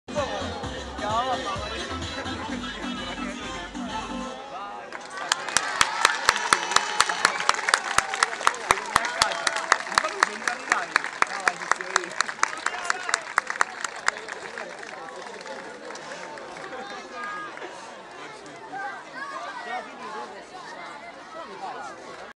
XIII Memorial Giuliano Di Donato 1
Poggio Moiano, 12 agosto 2015, Las Estrellas si esibiscono prima della finale femminile